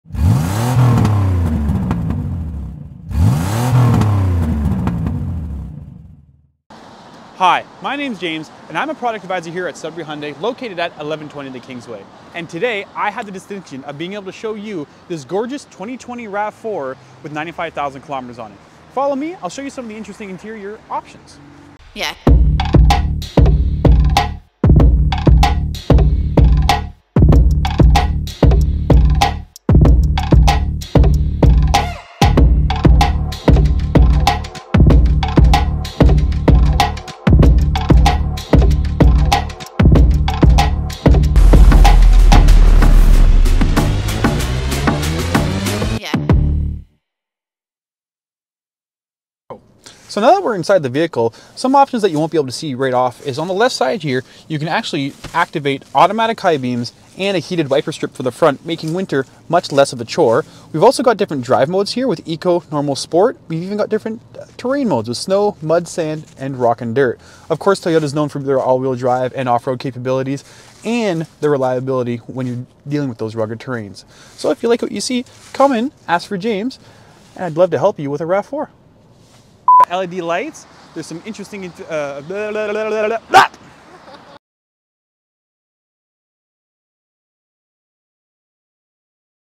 2020 RAV 4 - Feature Highlight with James
In stock now at Sudbury Hyundai is this 2020 RAV 4, meticulously well taken care of.